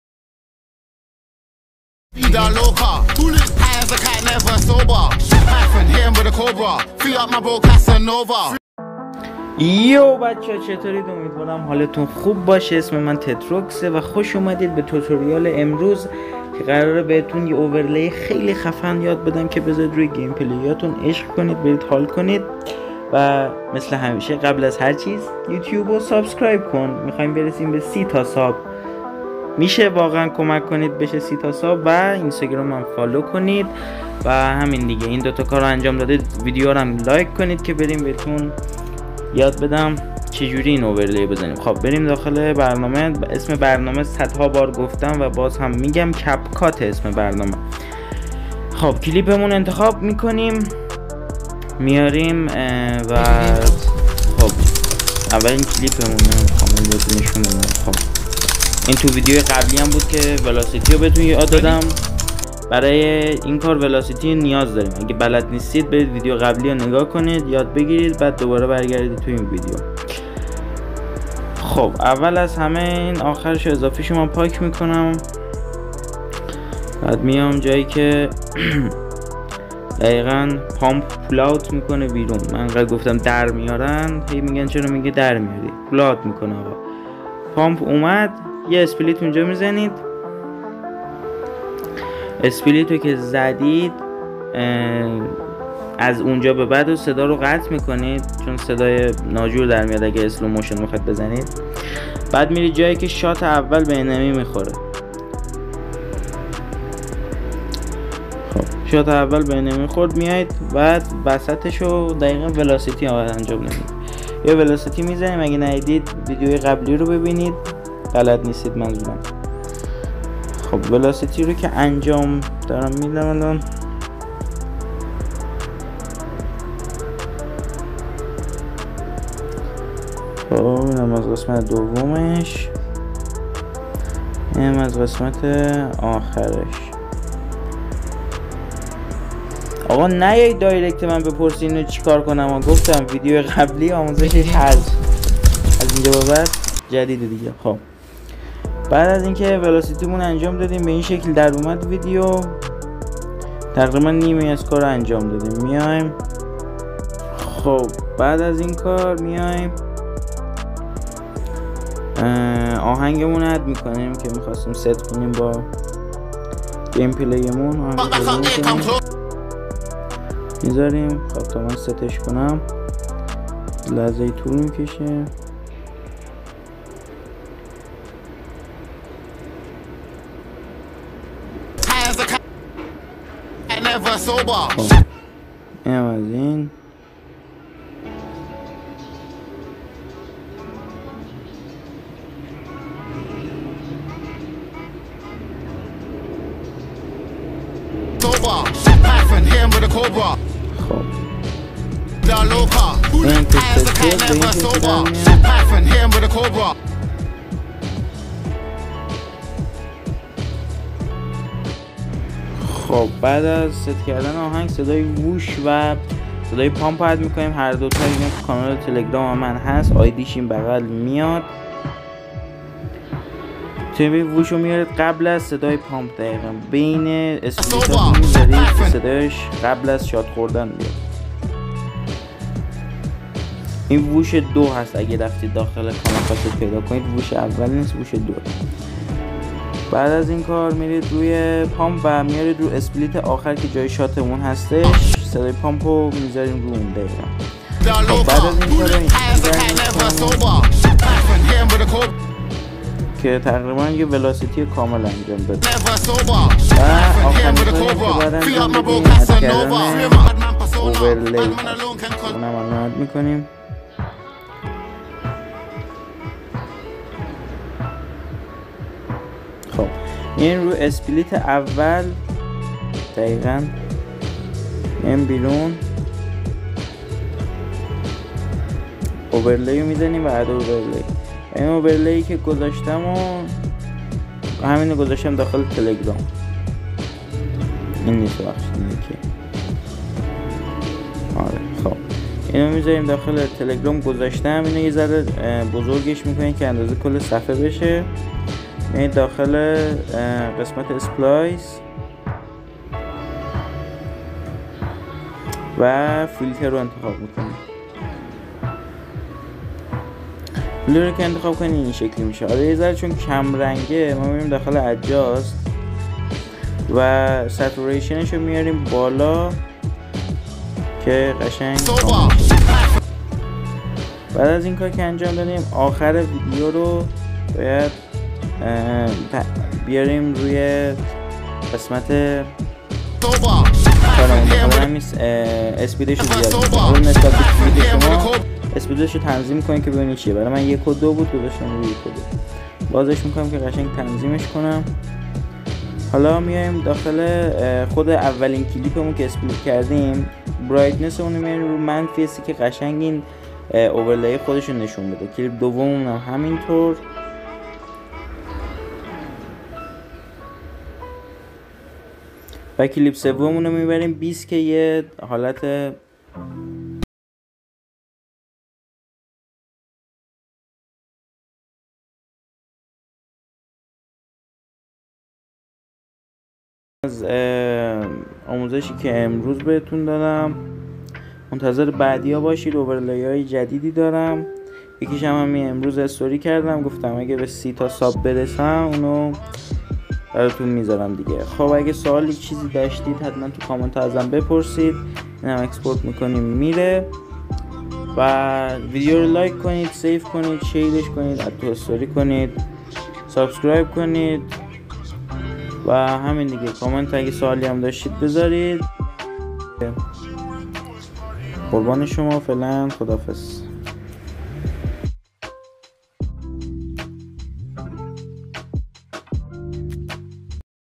How to: Split Screen Effect (@Avxrage Classic) on iPhone/android for *FREE* | iPhone/android Edit
Apps used: (FREE)
cap cut